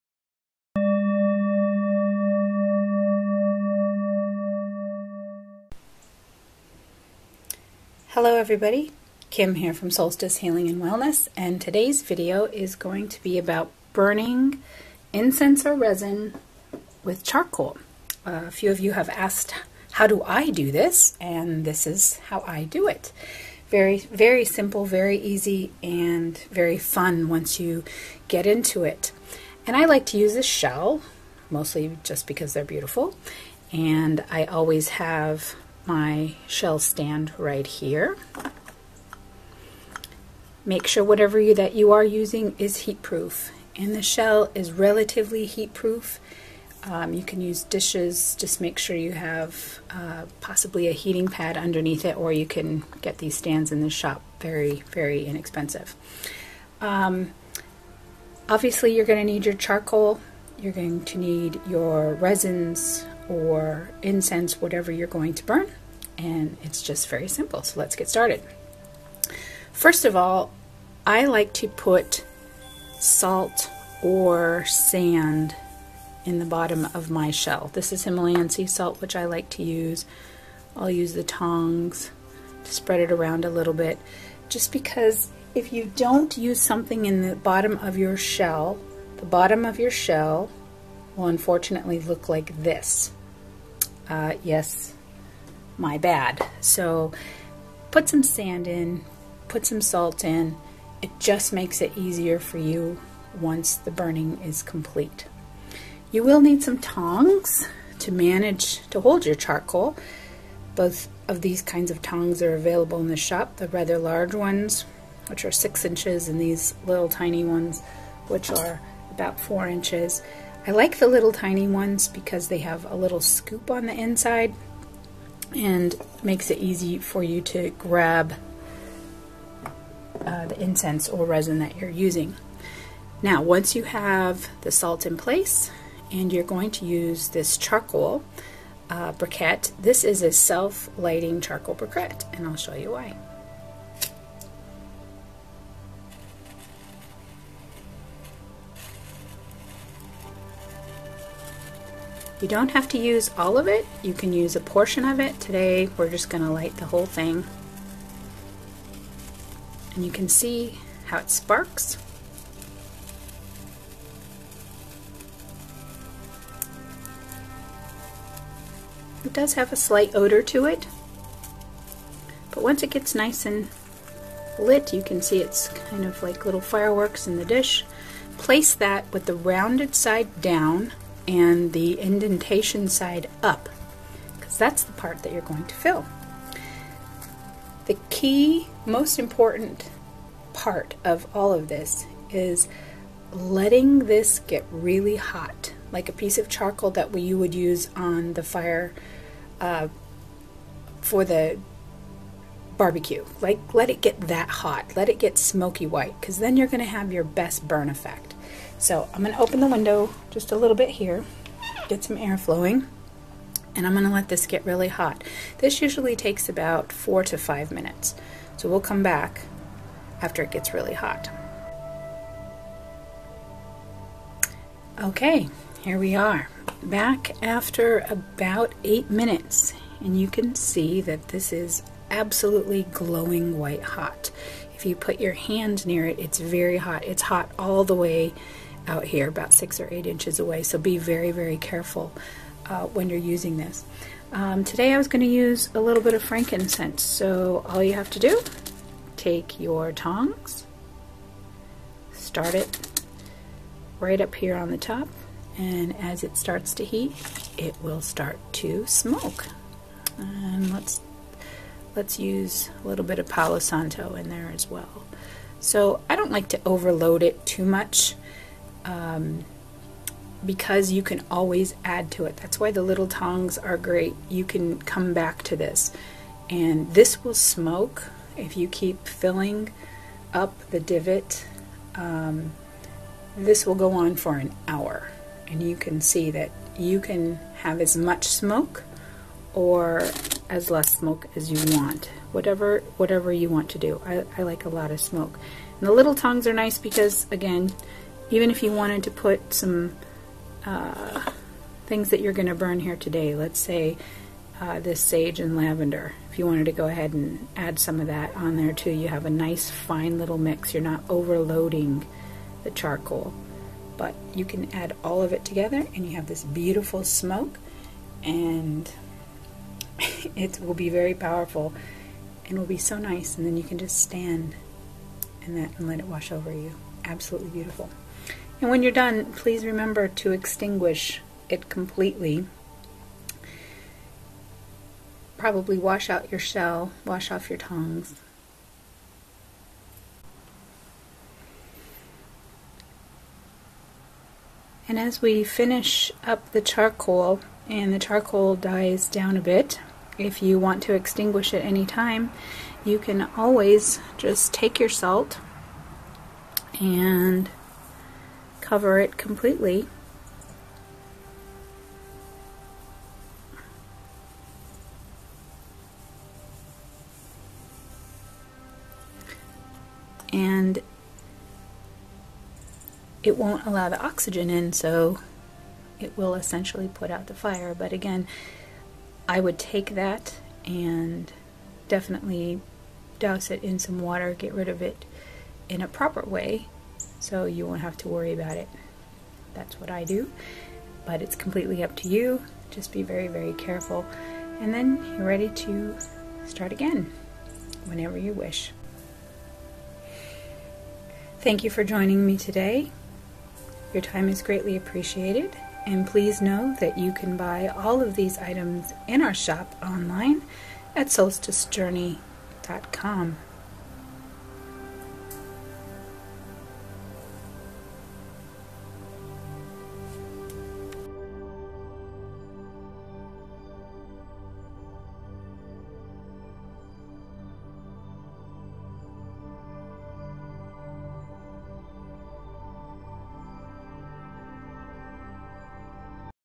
Burning with Charcoal
In this video, I will be showing you how I use a charcoal disc when burning resins or botanicals for smudging.  I use a charcoal disc when burning incense for extended periods of smudging or if I desire lots of smoke.  There is no re-lighting of the charcoal as in a smudge stick, just a continuous addition of incense.  One disc of charcoal can burn for up to an hour, in this video, I teach you how I choose to burn incense with charcoal.  Please use caution and extinguish properly when finished.

Supplies in this video:
Burning dish, I use a large Abalone shell and a 4-inch wooden cobra stand
Tongs, I am using 4-inch tongs
Charcoal discs, coconut husk charcoal also available
Salt, I am using Himalayan
Resin, I am using Copal
Herbal blend, your choice of herbs; I am using Lavender, Palo Santo and Copal Resin